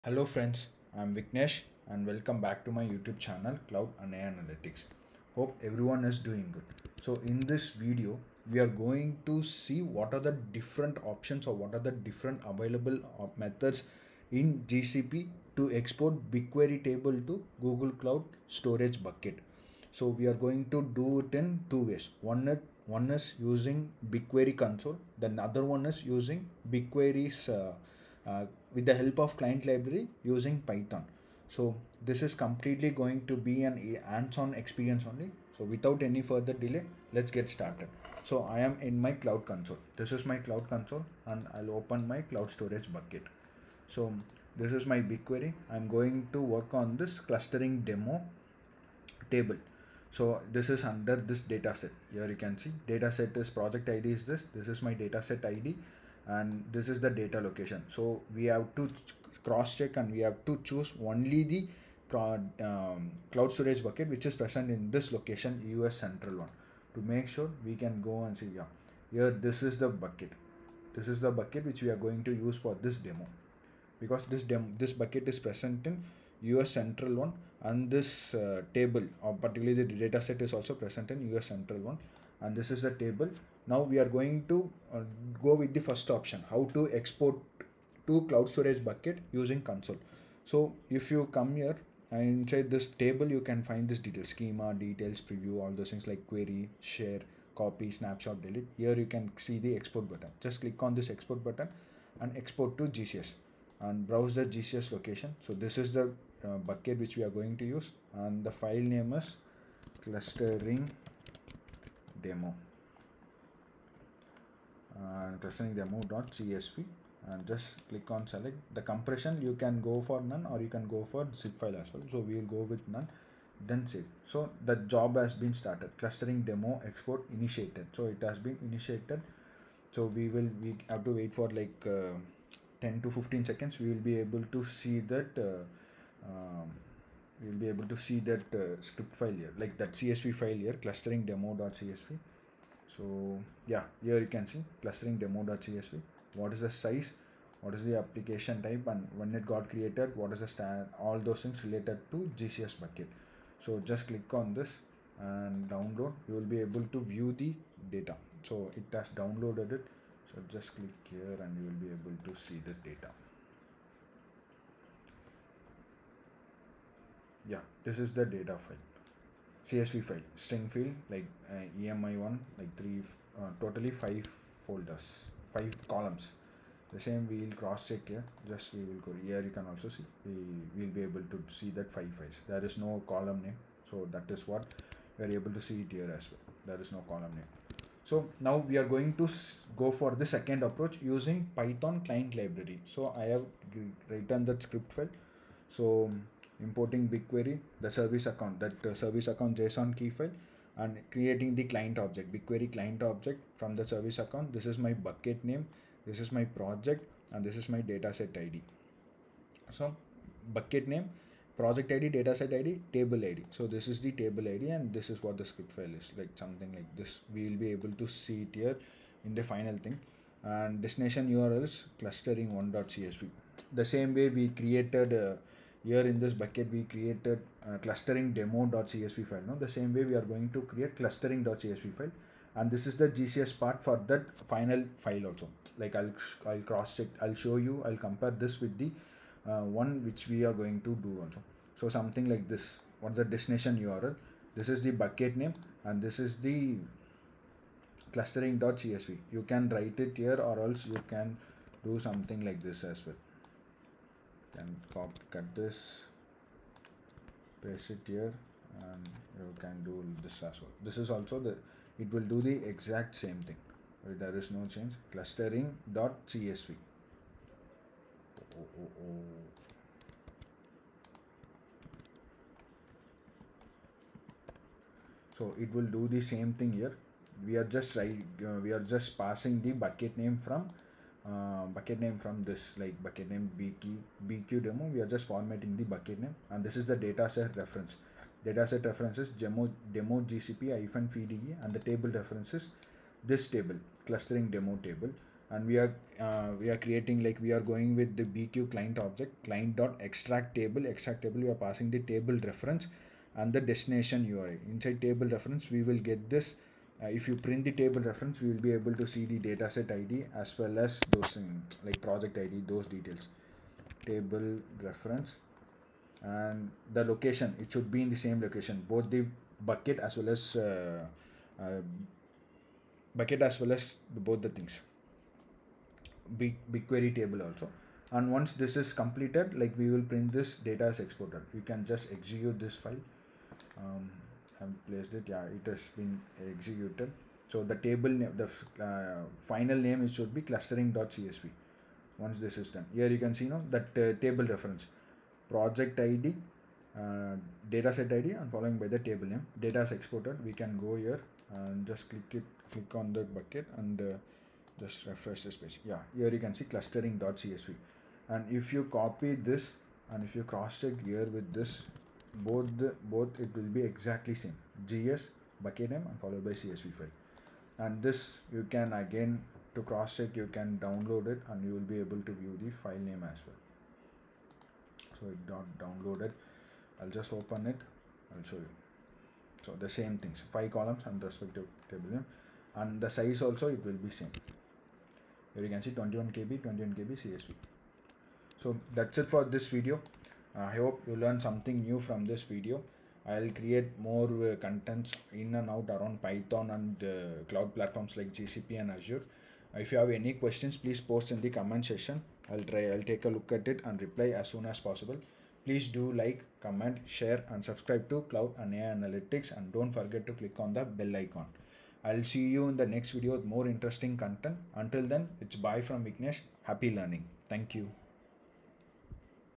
How to export BigQuery Table to Cloud Storage bucket in GCP
Services Used GCP:

1. Cloud BigQuery
2. Cloud Storage
3.  Python

Different ways to export BQ table to GCS bucket:

1. using BigQuery Console
2. using  Client library - Python

1. LIKE
2. SHARE